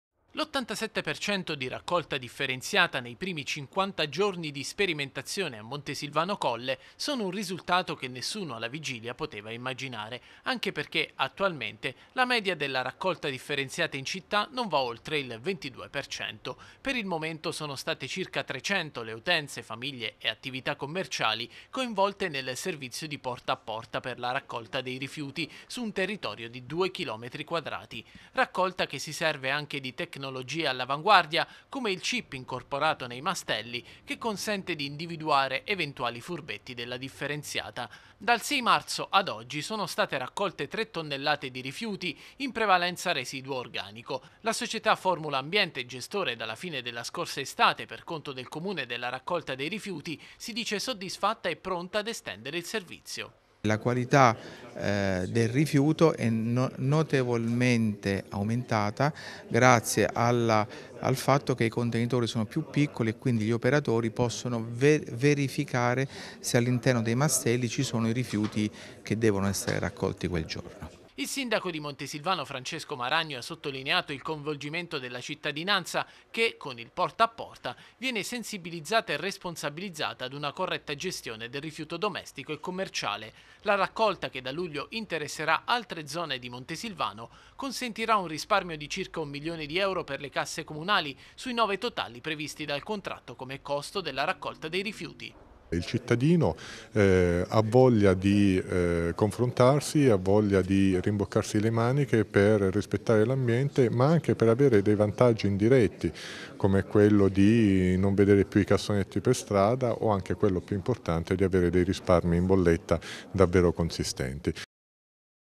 Montesilvano Colle all'87% di differenziata
Ottimo risultato per la sperimentazione del porta a porta a Montesilvano Colle. Il servizio di raccolta dei rifiuti, a breve, sarà esteso anche ad altre zone di Montesilvano